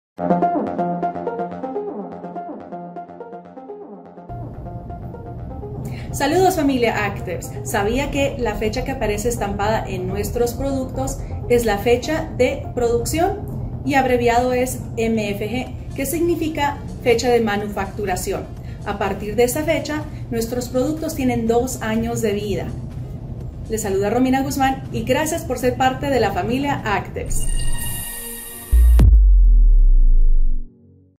ACTIVZ TIP Fecha de Manufactura impresa en el producto
Familia ACTIVZ: Este es un Tip muy importante, ¿sabías que puedes verificar la fecha de manufactura del producto solamente viendo el fondo del frasco?

Esto es de gran utilizad para asegurarte de que siempre tengas el producto más reciente.

Términos Relacionados

- ACTIVZ
- GNM-X
- ACTIVZ Tips